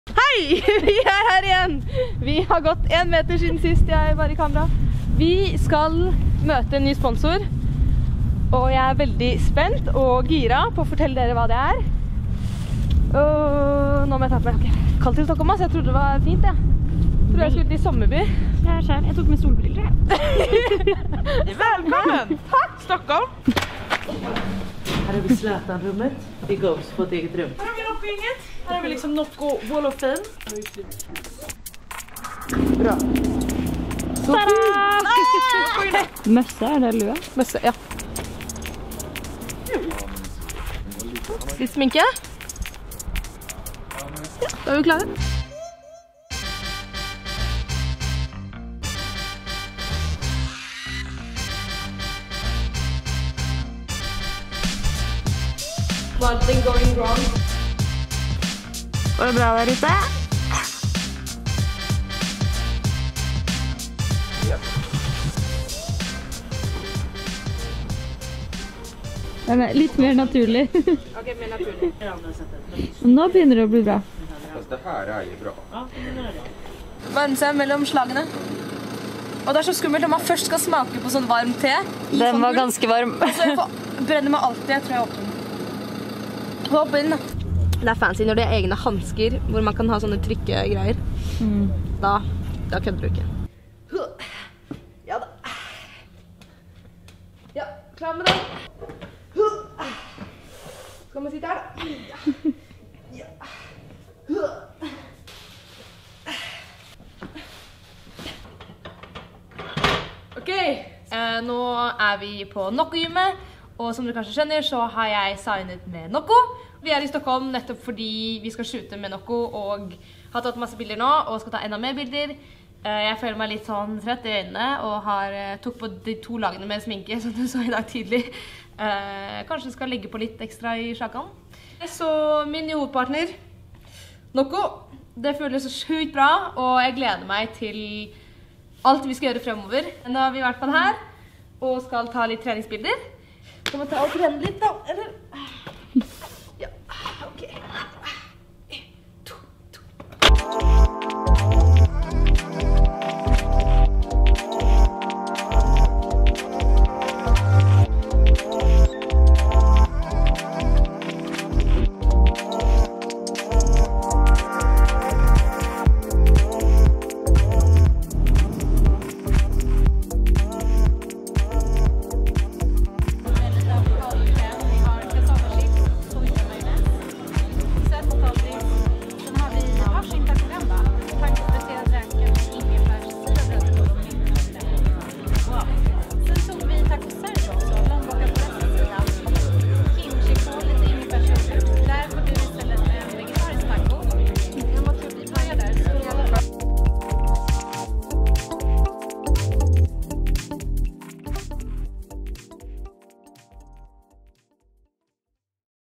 VLOG 45: NEW PARTNER - NOCCO
I'm super excited to announce my new partner - NOCCO!!  NOCCO is a sugar free functional beverage from Sweden, and will give me loads of energy this season! This vlog is from our first shoot, and you can learn more about NOCCO

Camera: GH5 with Leica 12-60mm

Filmed and edited by: Lea Hilden